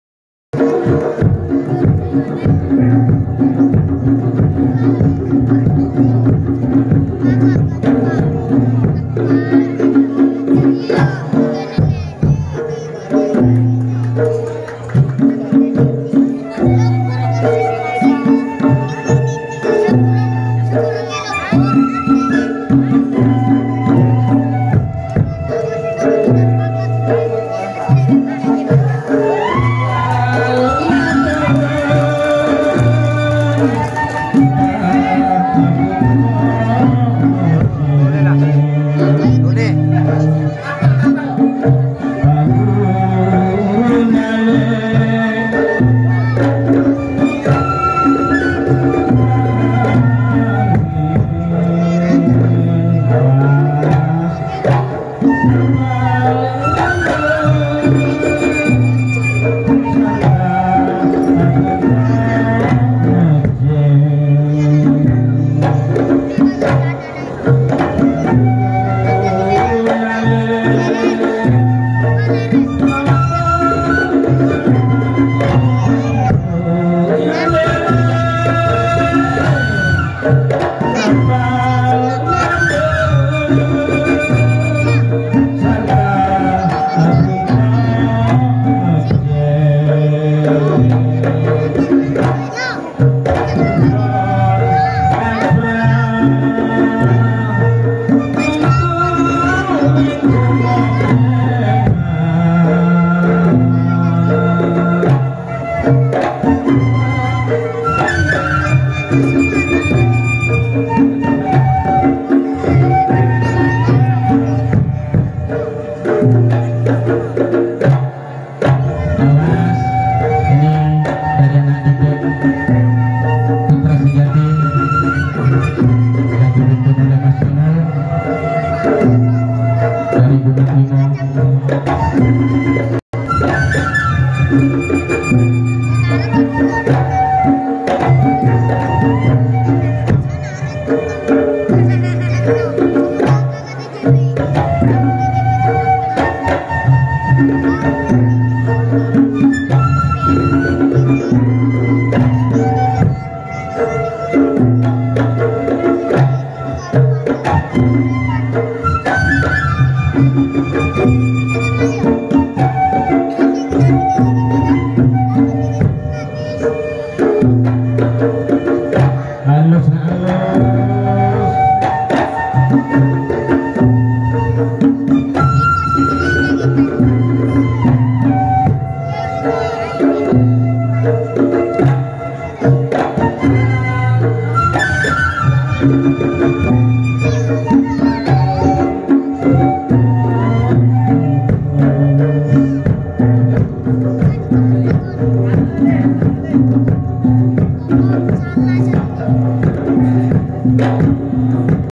🔴Padepokan silat Cimande bela diri‼️seni Jawa timur Indonesia Nusantara 9 Januari 2022
Salam Cimande Jember Jawa timur Indonesia Nusantara salam persaudaraan dari kami tjimande PUTRA SEJATI bersama putra desa dari kota Jember.